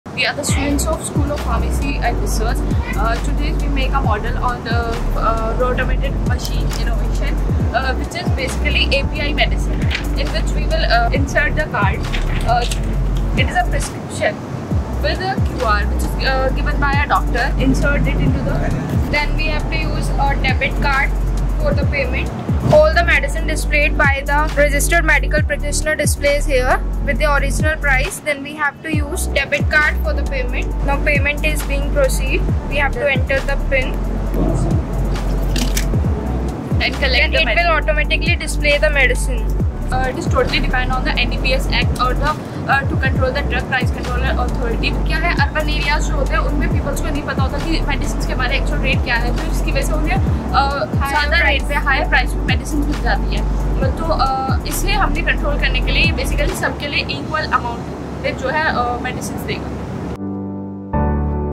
Medicinal API (Automated Pharmacy Innovation)
One Person, One Dose: Award-Winning Innovation Fights Drug Abuse & Streamlines Healthcare!

The future of pharmacy is here!  Our Medicinal API (Automated Pharmacy Innovation) is a revolutionary concept that won first place at the Navadhara 2024.

This cutting-edge system combats illegal drug trafficking and ensures fair medicine pricing. But that's not all!  During pandemics, Medicinal API promotes social distancing with its "one person, one use" approach. ️